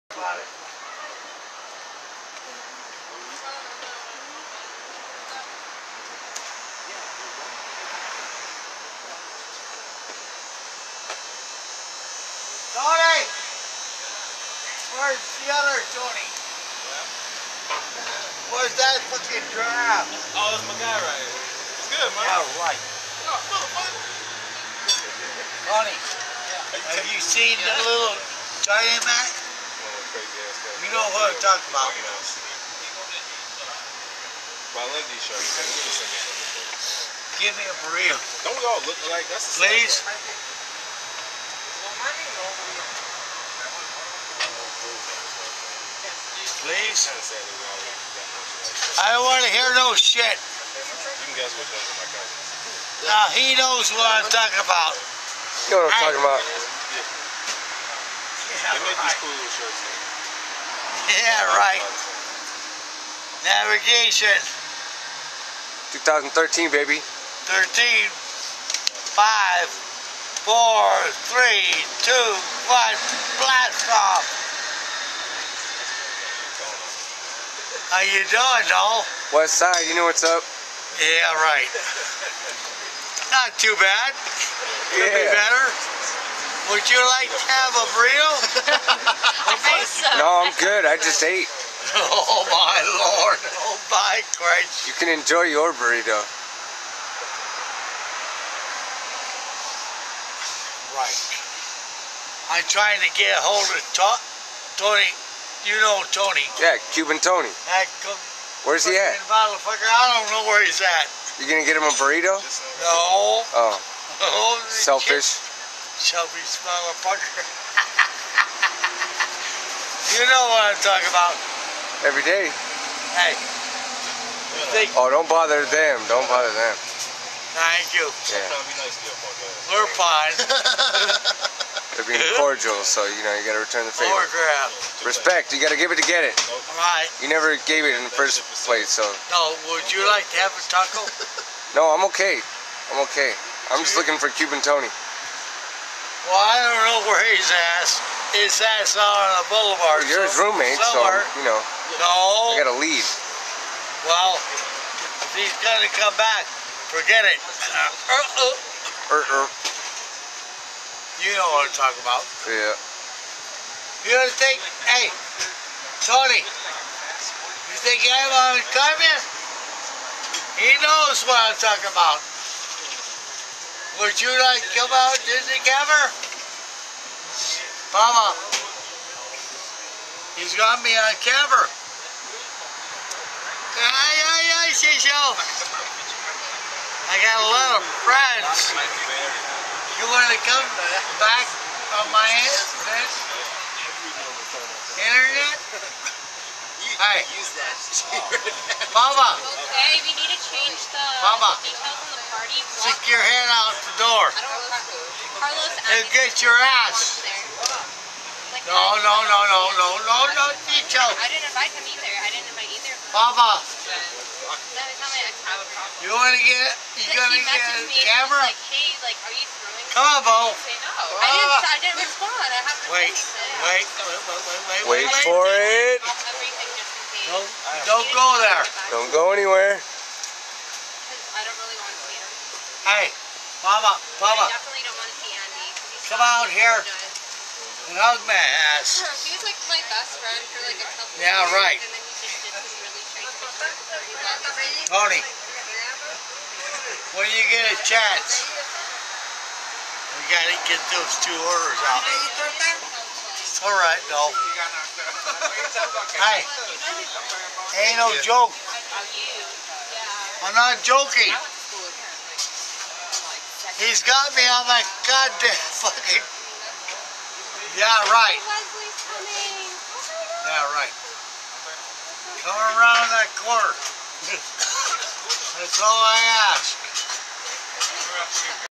He Knows What I'm Talkin' About
Jerry demands a burrito from the Taco Truck in Old Town Pasadena and continues to tell me that I know what he's talkin' about...